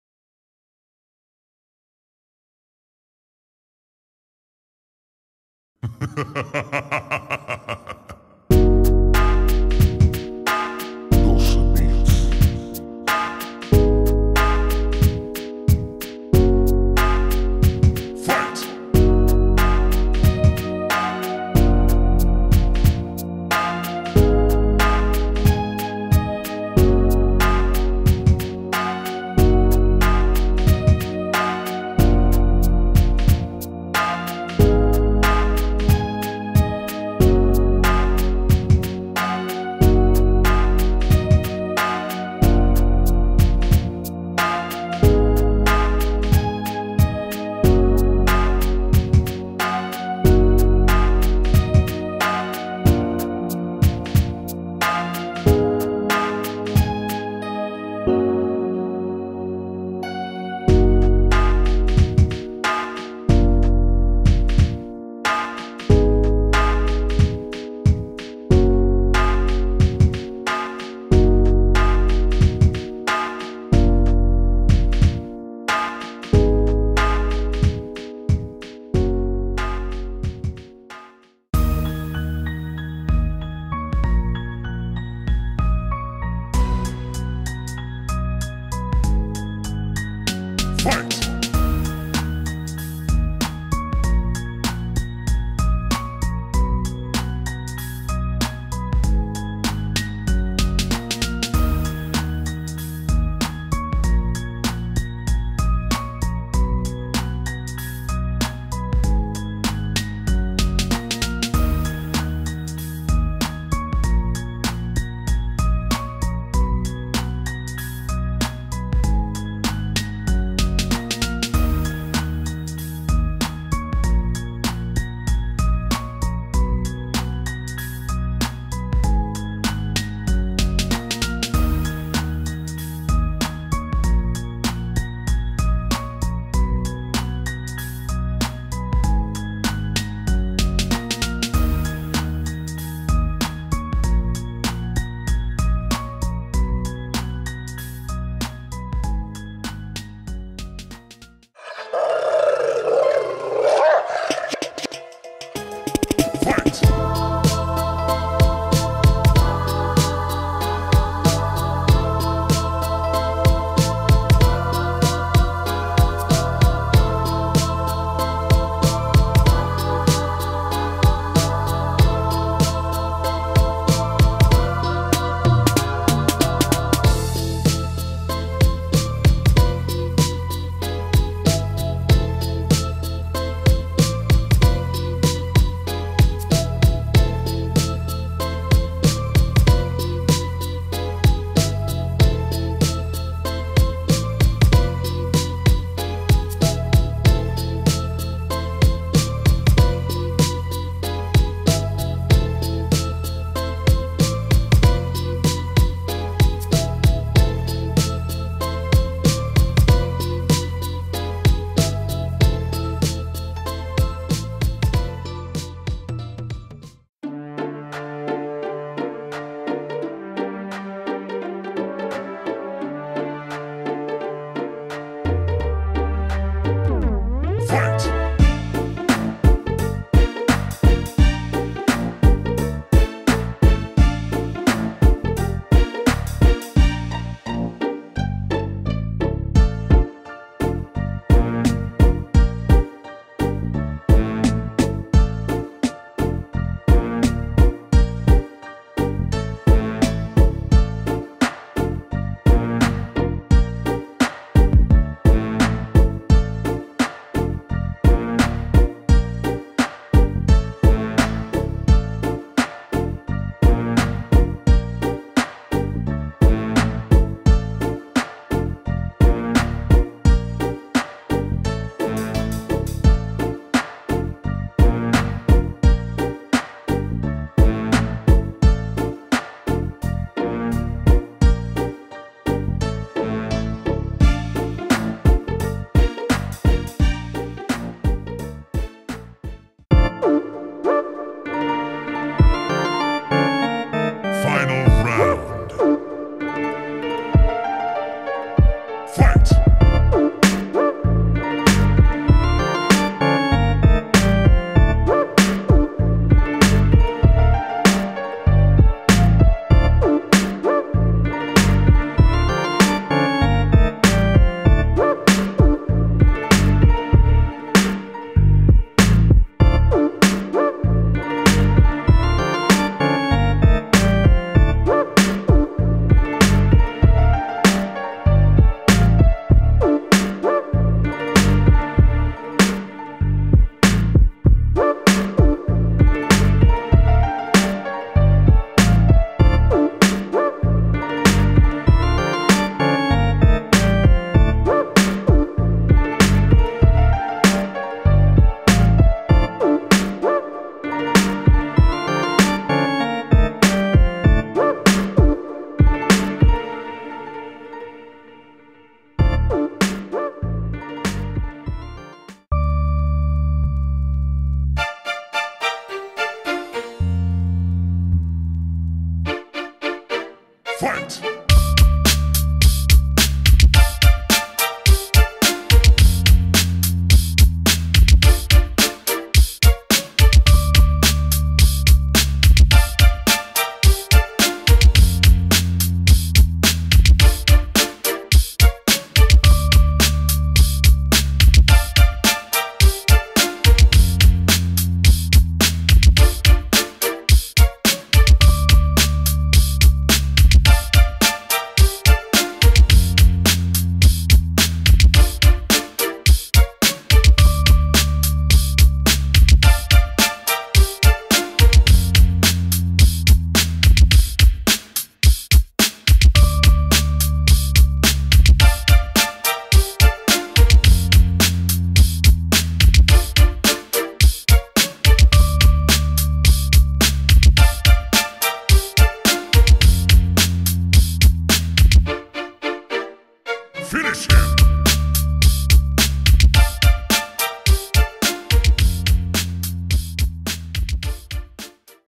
BATALLA DE FREESTYLE Nro 9 CAMBIA DE PALABRAS CADA 20 SEGUNDOS !!Que gane el mejor¡¡, hiphop
DESCARGA LOS BEATS DEL VIDEO

.SLOW DOSE.
.RAP DIFERENCIAL.
.COLOMBIA.